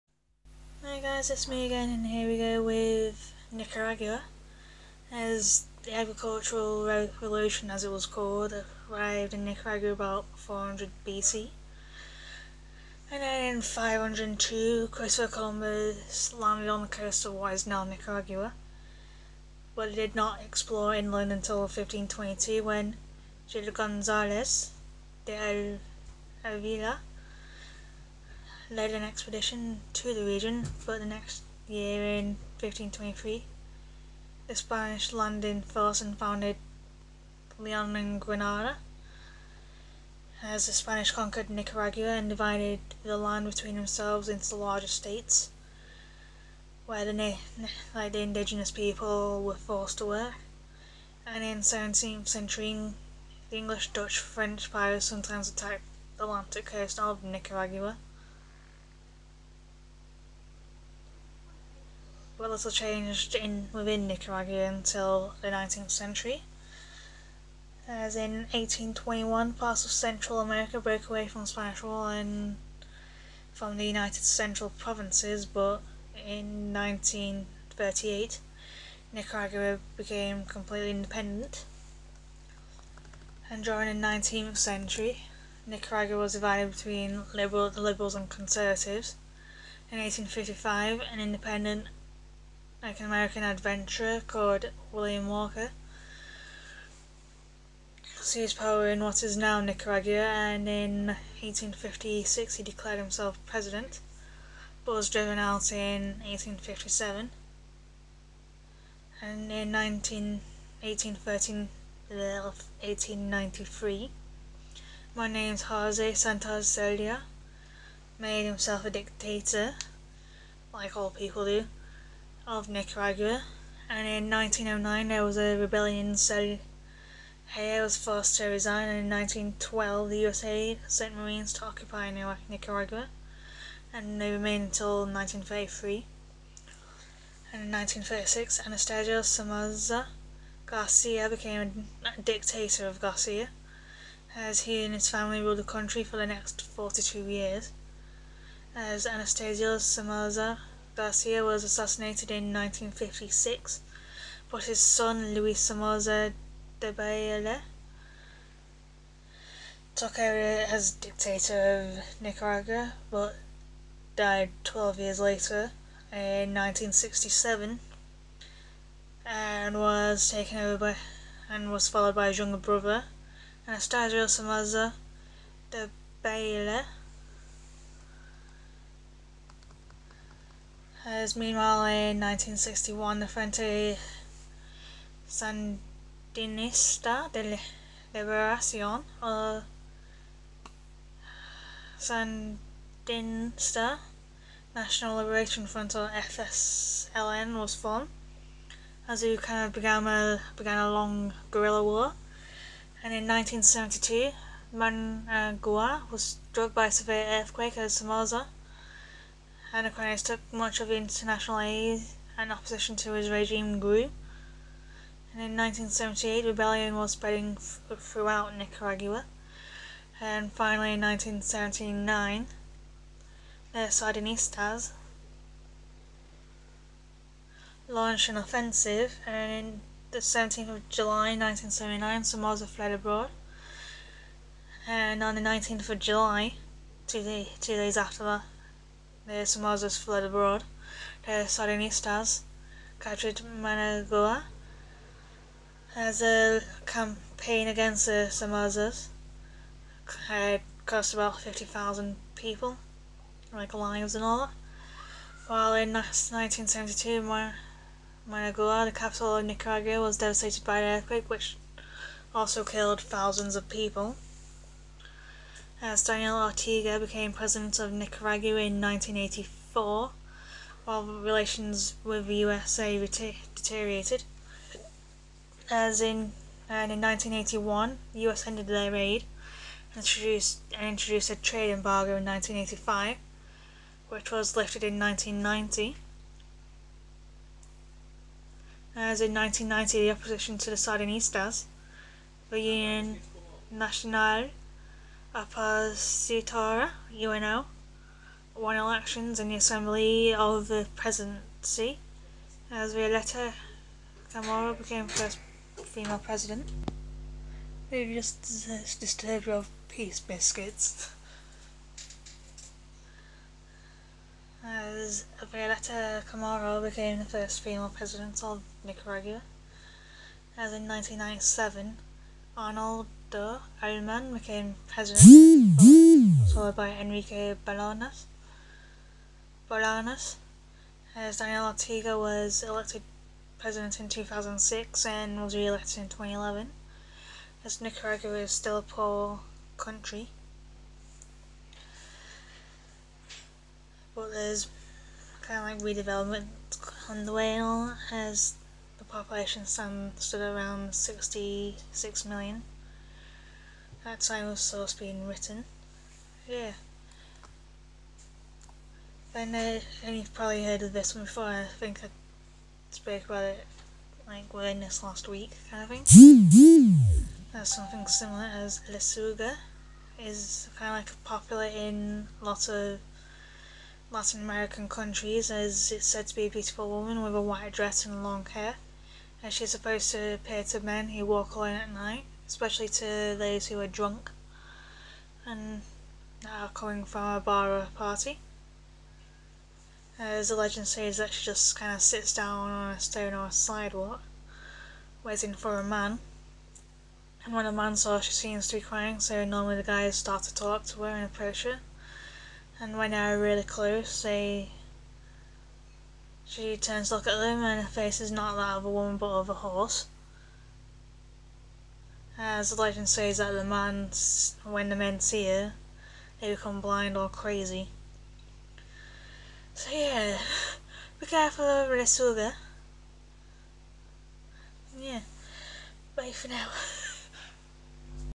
Nicaragua
A small country country that has had intermittent dictatorships and natural disasters, it has not had it easy. Yet there is more to the country.